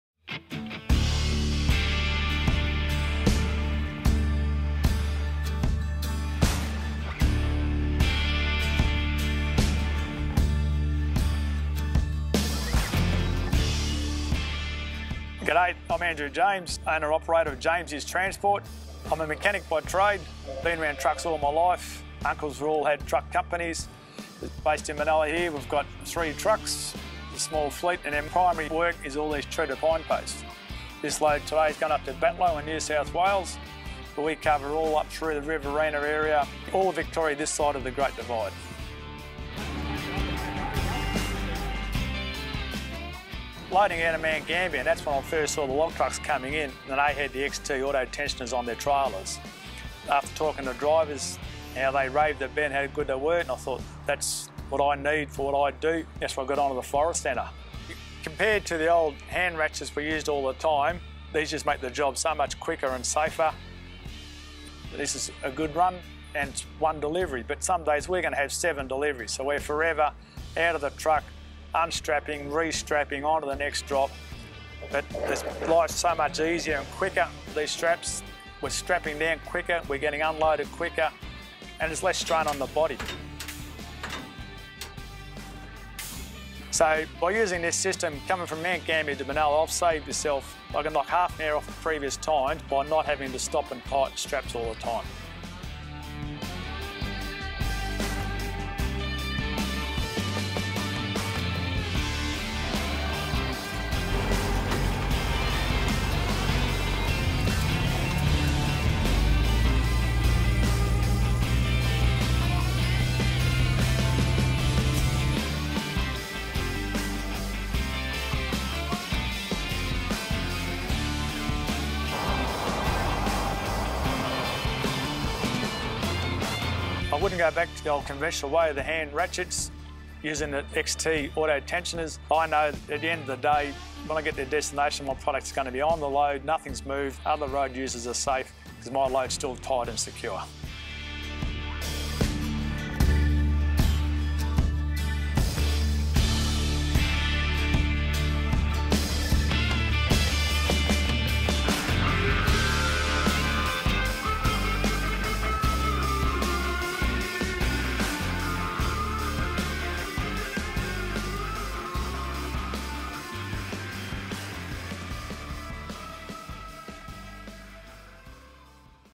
Hauling timber posts - ExTe air operated load restraint
Owner of Jamesy's Transport, Andrew James shares his experience trucking using ExTe air operated load restraint.
Hauling timber posts, and also hay bales and concrete troughs, most of Andrew's cargo settles during the journey - which would normally require extra time and effort checking and re-tensioning straps.

Trailer built by Pumpa Manufacturing - Pumpa.net.au
ExTe TU auto-tensioners supplied in Australia by Forest Centre - Forestcentre.com.au